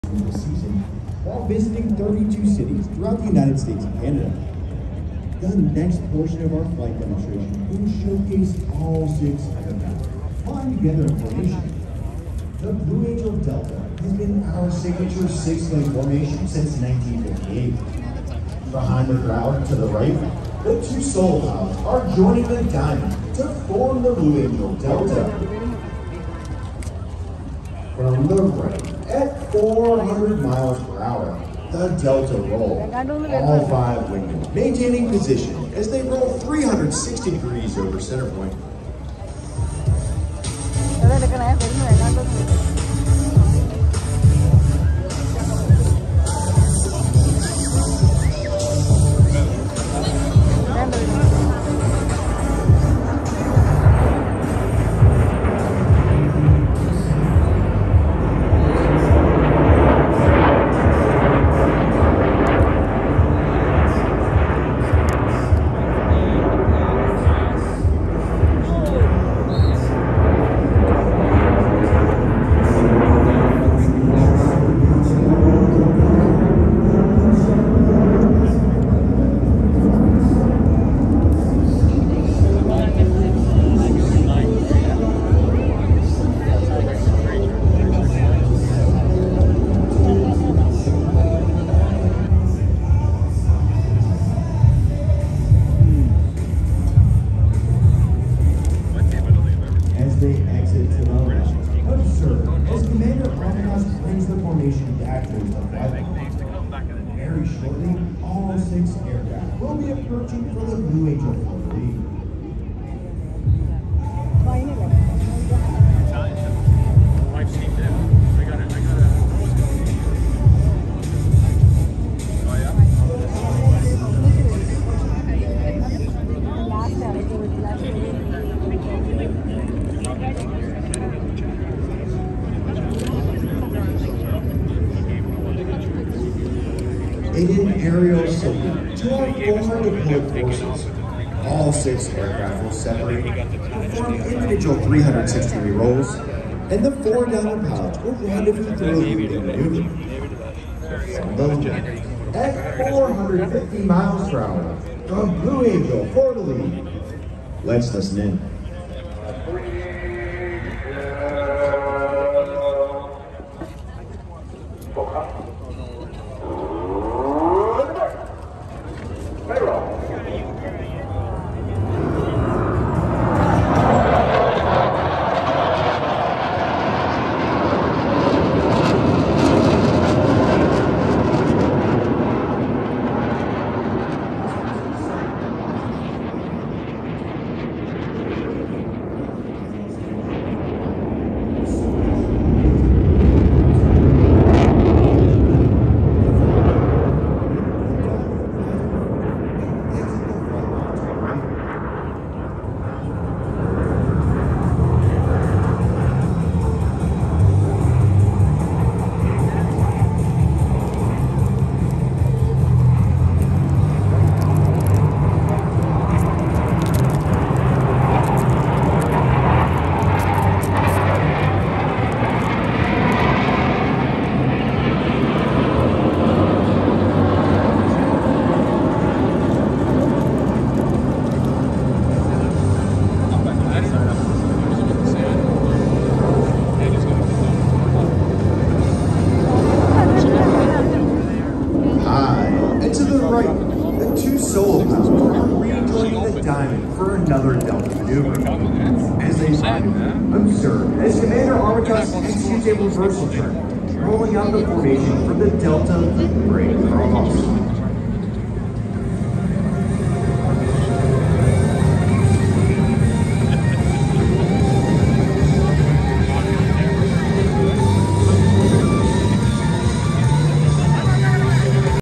The best spot where to watch the Canadian International Air Show II Labour Day 2023
The Canadian International Air Show (CIAS)
celebrates its 74th year with an incredible aerial extravaganza!

The Exclusive Air Show Zone (1095 Lake Shore Blvd. West) is located directly in front of center stage and has live narration of the event playing throughout. The show can also be seen from some areas within the Canadian National Exhibition (CNE) Fair